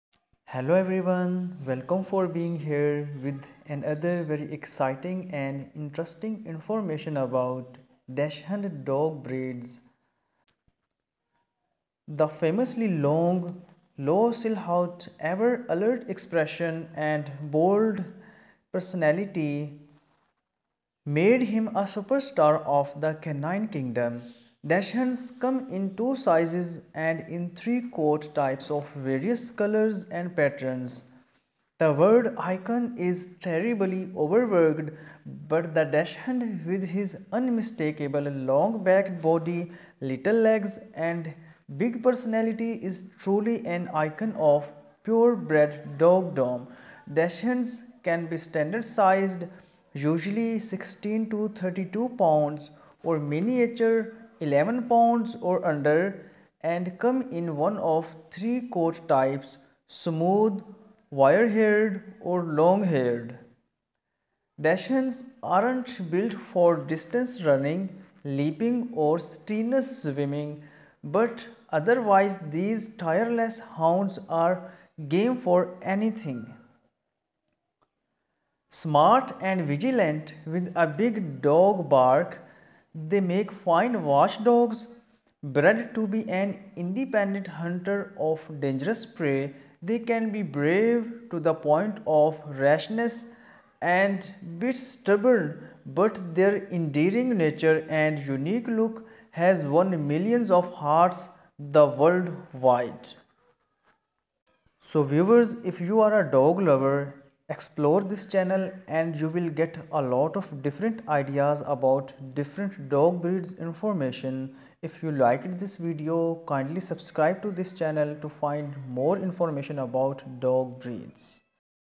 Everything You Need To Know About Dachshund Dogs Breed | Mini Dachshund Dog Breed Facts | Cute Dogs
Dachshunds are scent hound dogs bred to hunt badgers and other tunneling animals, rabbits, and foxes. Hunters even used packs of Dachshunds to trail wild boar. Today their versatility makes them excellent family companions, show dogs, and small-game hunters.

This small, drop-eared dog is tough enough to take on a badger. In fact, that’s how they got their name (Dachs meaning badger; hund meaning dog).